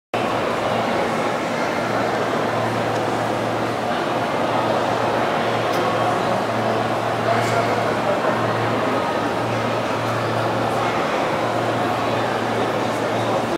John the Baptist's head
The shrine containing John the Baptist's head, allegedly, in the Umayyad mosque in Damascus.  He is considered a saint also by the Muslims.